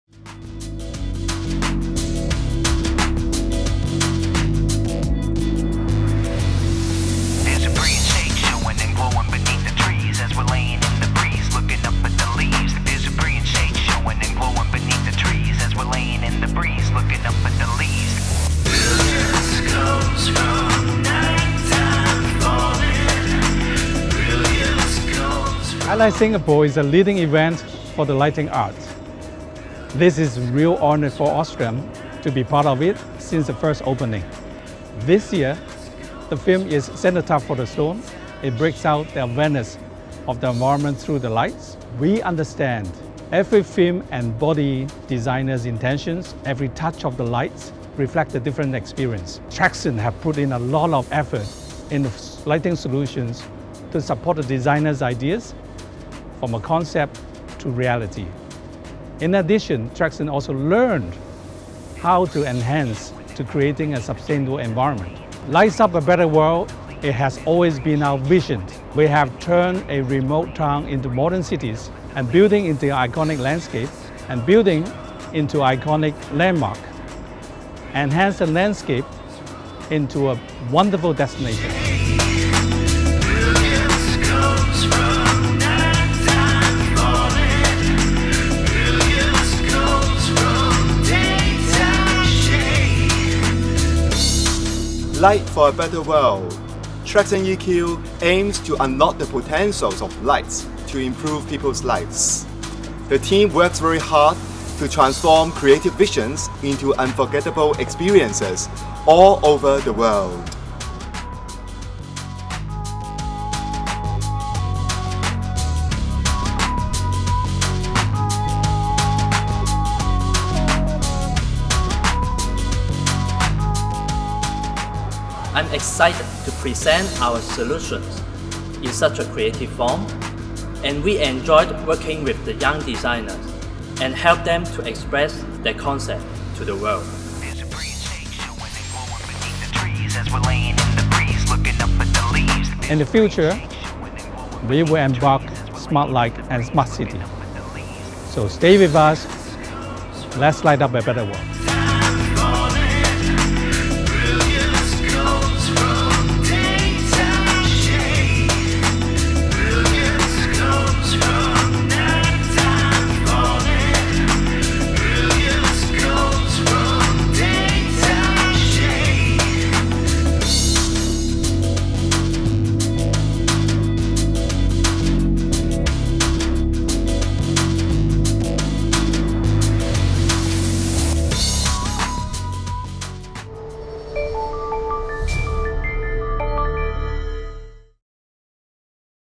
I light Singapore "Cenotaph for a stone" - Singapore
Traxon e:cue joined hands with young designers to participated in the i Light Singapore 2019 sustainable lighting art festival, advocating environmental protection and sustainable development.

i Light Singapore, the leading light art festival rooted in Singapore and known worldwide, has been held for six times. Upon the 200th anniversary of the Lion City’s port opening, the 2019 version was renamed to “i Light Singapore - Bicentennial Edition”. The Marina Bay once again enchanted visitors and citizens with magnificent brilliance.

28 nights, 33 art pieces and 5 program centers…this year’s festival has drawn thousands of visitors. All these marvelous installations were put together by recyclable material and lit by energy-saving illumination technology. They were embodiments of environmental concept, aesthetic taste and humanistic philosophy, facilitating conversations between the designers and people across the world.

Cenotaph for a Stone is such a work that realized the dialogue traversing space and time. Inspired by the Singapore Stone, Bryan Joseph Cadag, Loo Quan Le and Zulkarnain Bin Mohd Zin, the young designers from University of Singapore, created this masterpiece.

The installation comprises 51 ‘rock fragments’ on steel pedestals, with the 52nd pedestal left empty, hinting at the only piece that remains of the Singapore Stone residing in the National Museum of Singapore. The ‘rock fragments’ are placed in an orchestrated fashion that hypothesizes what the stone could have looked like. Traxon e:cue added glory to the Cenotaph for a Stone with unique sustainable lighting solutions with the OSRAM ARCHISHAPE® 2.0 Dot and e:cue Butler S2 lighting control system.

Since day one i Light Singapore has been known as a sustainable event, which carries the theme of sustainability and showcases green-lighting art pieces. It aims to raise public awareness of environmental protection and a sustainable future across Asia and even the whole world.

The development of lighting source tells how the concept of sustainability evolves. From torches, candle and gaslight to incandescent light bulbs and LED luminaires, we become greener each day.